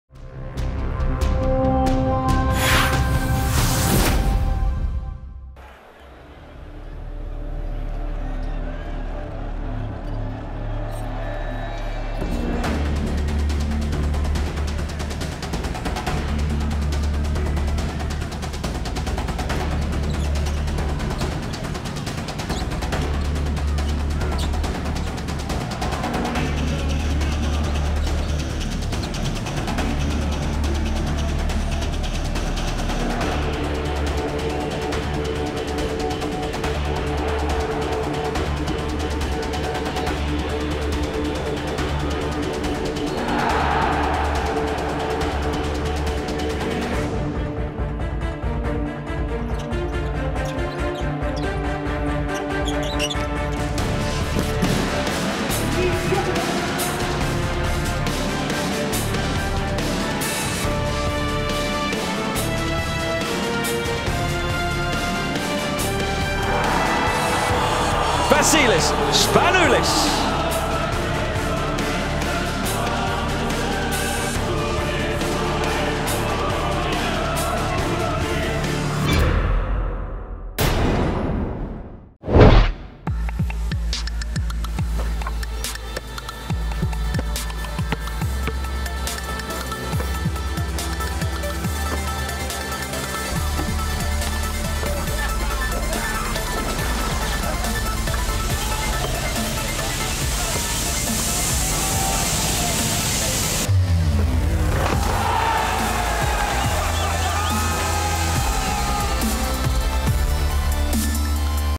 All-Euroleague Second Team
Just before the basketball world turns its attention to Milan for the 2014 Turkish Airlines Euroleague Final Four, Euroleague Basketball is pleased to honor 10 of the stars that made this season so spectacular by releasing the 2013-14 All-Euroleague First and Second Teams.

The All-Euroleague Second Team consists of Ricky Hickman from Maccabi Electra Tel Aviv, Vasilis Spanoulis of Olympiacos Piraeus, Victor Khryapa of CSKA Moscow, Nikola Mirotic of Real Madrid and Stephane Lasme of Panathinaikos Athens.